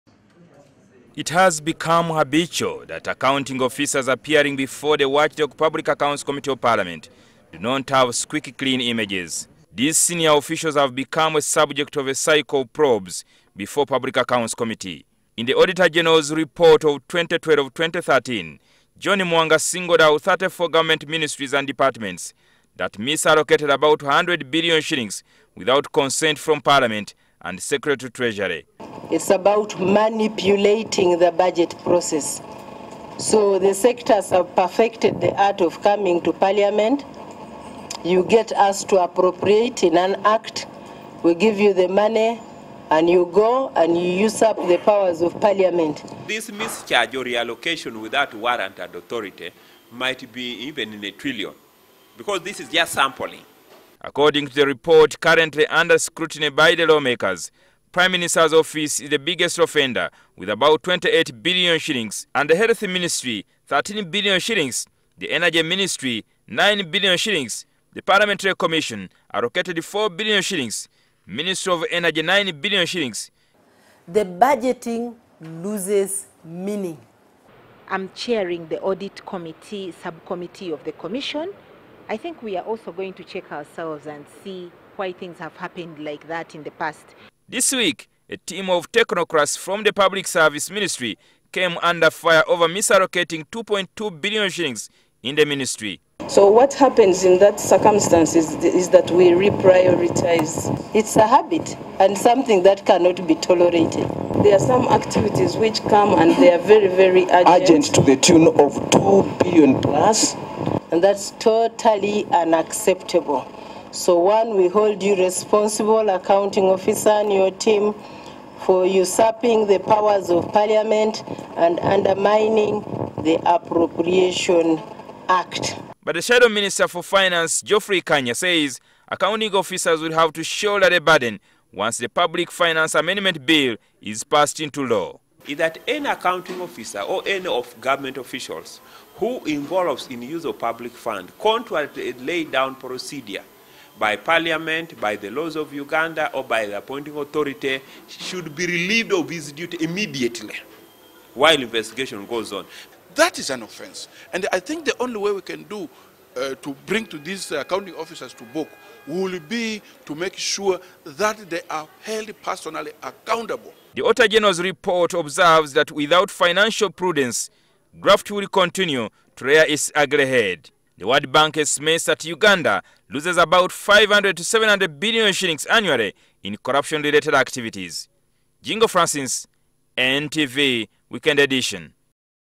Auditor General: Ushs100 billion misallocated by technocrats annually
The Auditor General, John Muwanga, has revealed that close to 100 billion shillings is misallocated from the budget by technocrats in government ministries and departments annually.